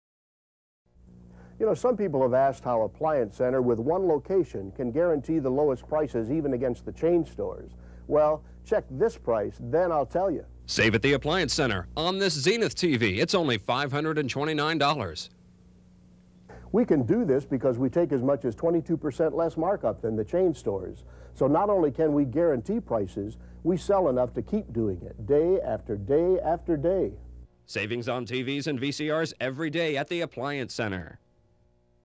Appliance Center spot 1983
We're celebrating 60 years with a look back at some of our classic commercials. This one hails from the early 1980s. Who remembers console TVs?